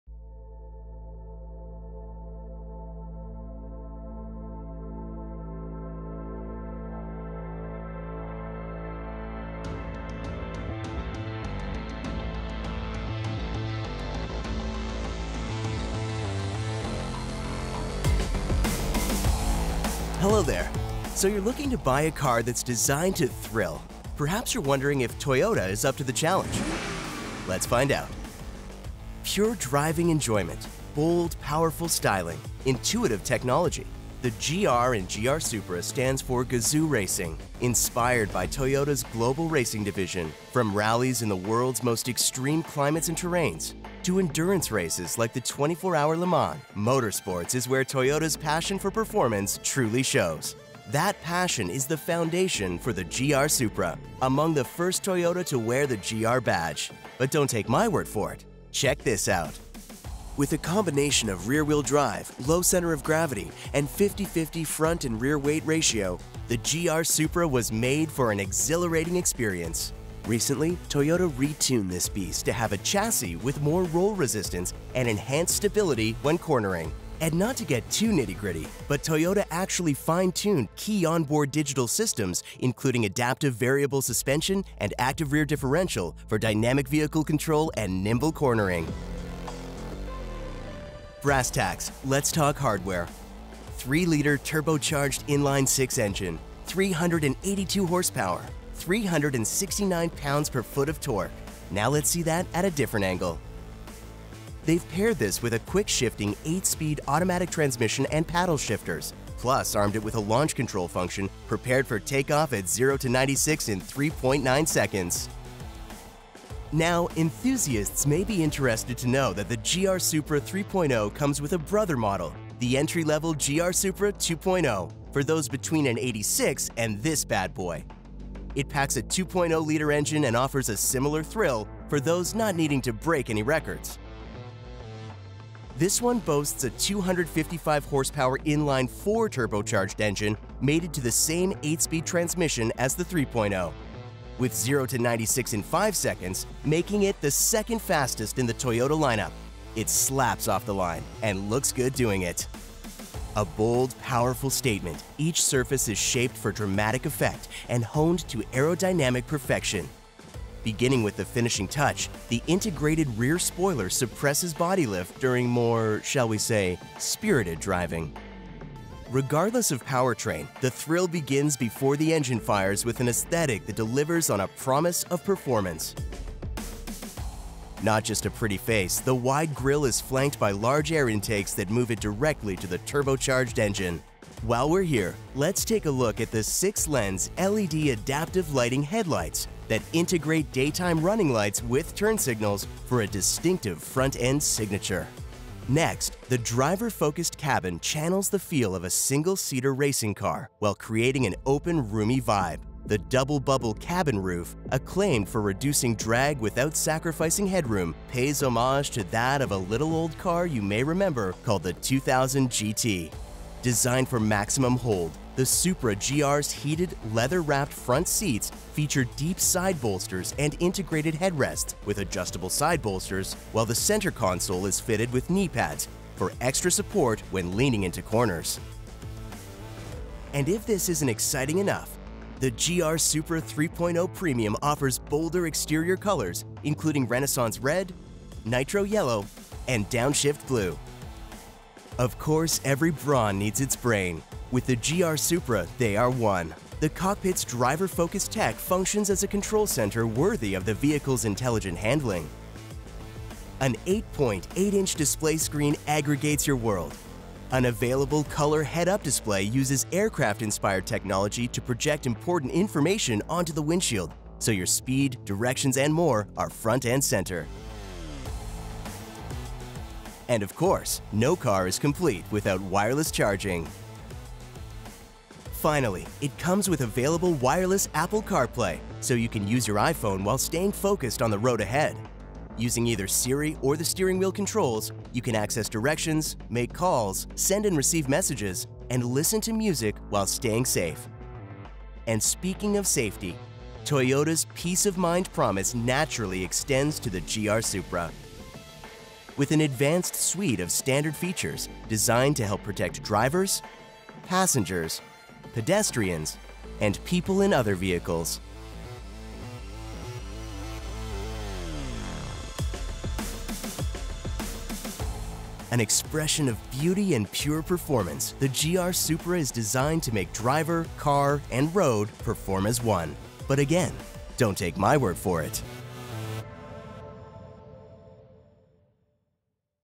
2021 Toyota GR Supra Overview
The Toyota GR Supra – it’s an expression of beauty and pure performance, designed to make driver, car and road perform as one.